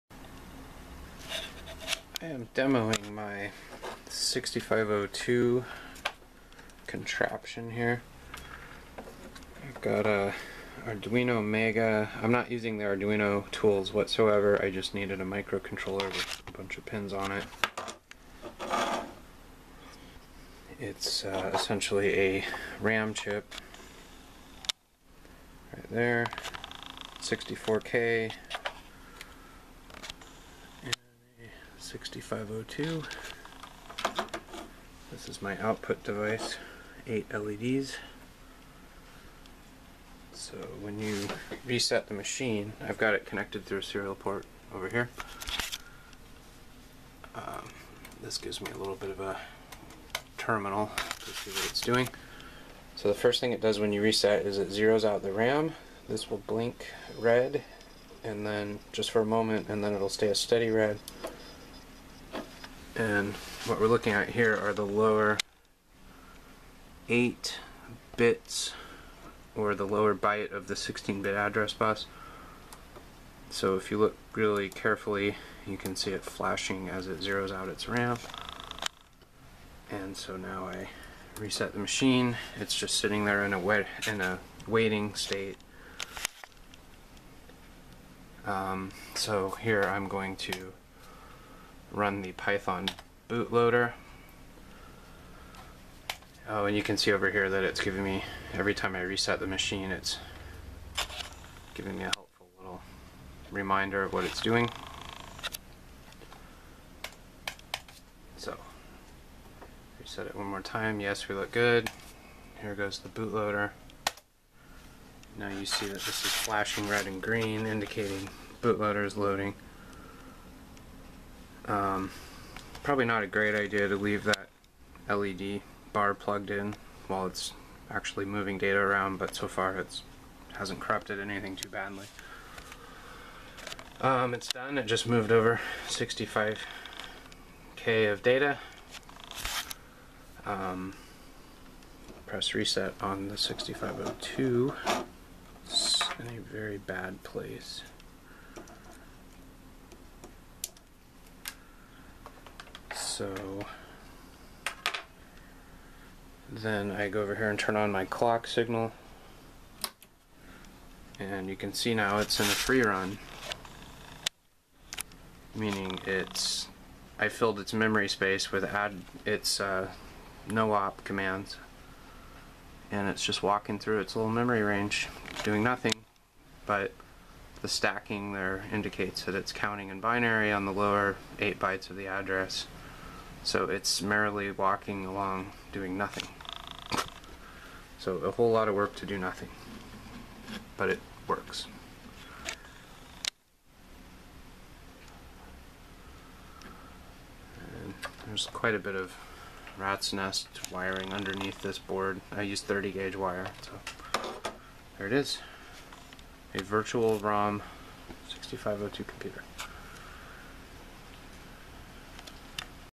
65c02 homebrew AVR
Demo of my third 6502 experiment. uses only an AVR chip, 65c02, and SRAM.  I bootload the code into 64K RAM to simulate ROM before clocking the CPU. Firmware written in AVR C, and python.